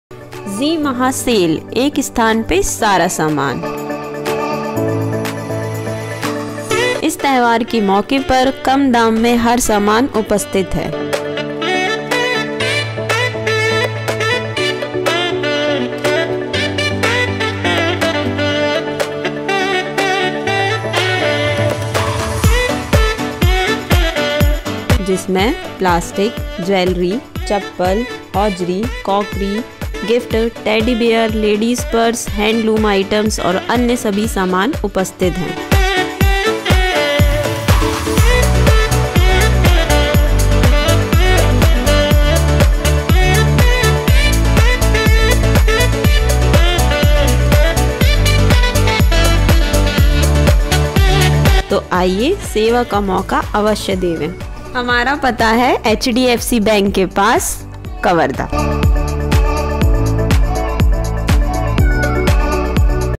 Zee Mahasale | Shopping Bajar Kawardha | DG World
Work Done on:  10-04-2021 For Pay Net Cable (Local City Cable)
Client Name  - Zee Mahasale
यहाँ आपको सभी प्रकार के सामान , कल्पना होटल के सामने कवर्धा
अगर आप भी अपने व्यापार का एड्स विडियो बनाना चाहते है| एकता चौक , कवर्धा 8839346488

Directed by: Md Salman

Cinematography :  Aadarsh Kesharwani